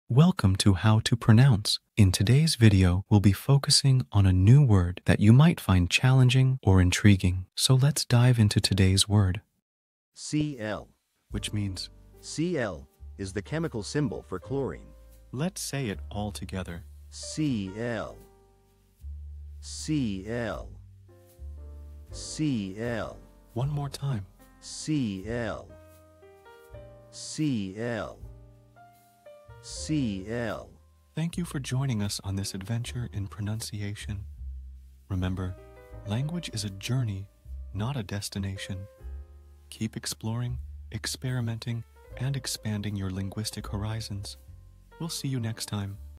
How to pronounce Cl
Master the Pronunciation of 'Cl' - which means : "Cl" is the chemical symbol for chlorine. 📝 with @HTPronounce 🎓🗣️ - [Your Guide to English]

Learn to pronounce 'Cl' perfectly in English with our easy and engaging tutorial by @HTPronounce! 📖📽️ In this comprehensive video, we provide you with native speaker examples 🗣️👂 and phonetic breakdowns 🎵 to ensure you get the pronunciation just right.

How to say Cl in English